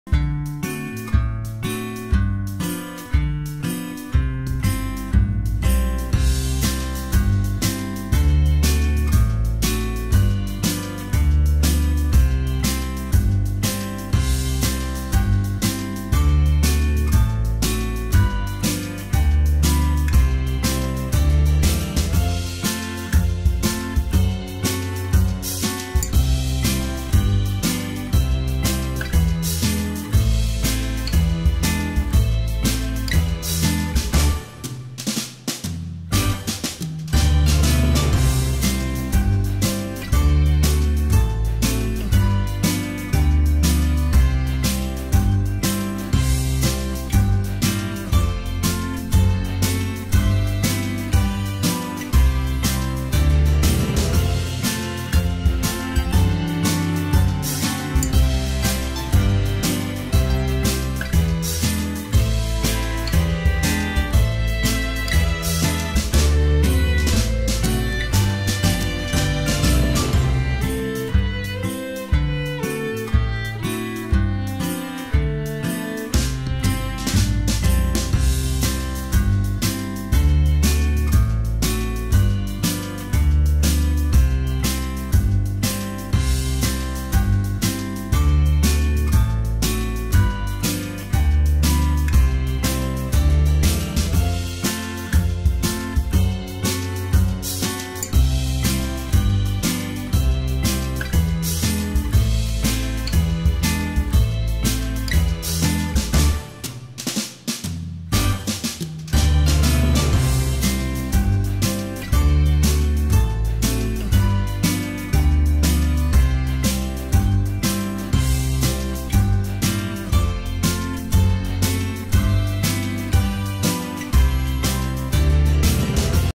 Máscara de Proteção com Meia / Protective Mask with Sock
Come learn to make this mask made with a sock !!! Easy and super comfortable !!!

Venha aprender a fazer essa máscara feita com uma meia!!! Fácil e super confortável !!!
Assine o meu canal e siga-me pintando ou fazendo muitos DIYs semanalmente.